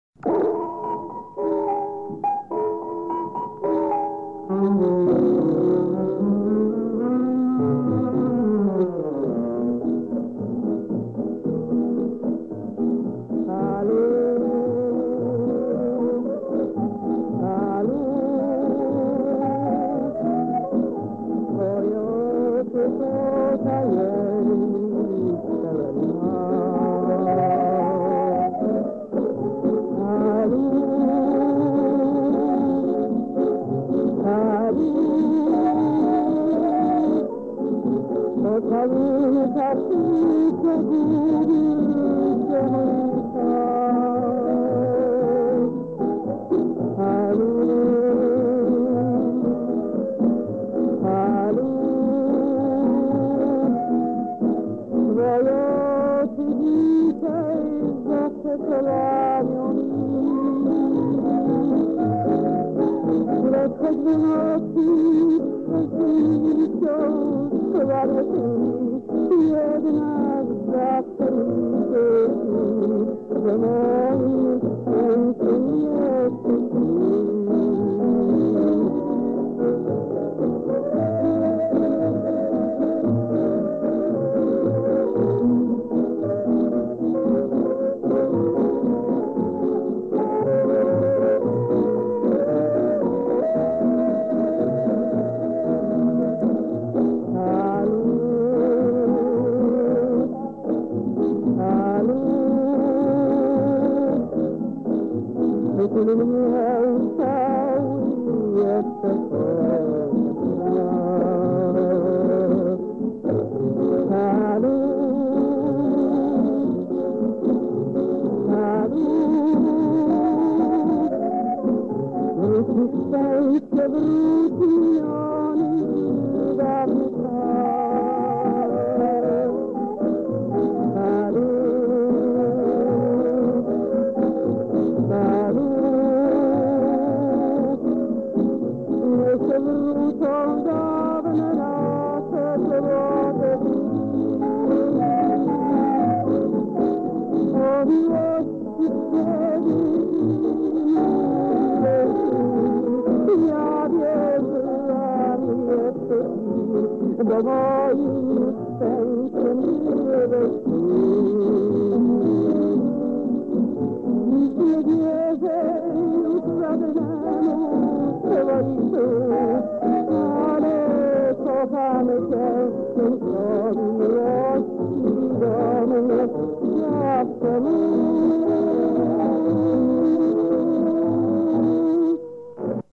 Palou.wmv
Oryginalny tytuł piosenki i jej autorzy do ustalenia.
Piosenkęśpiewa Vera Gran.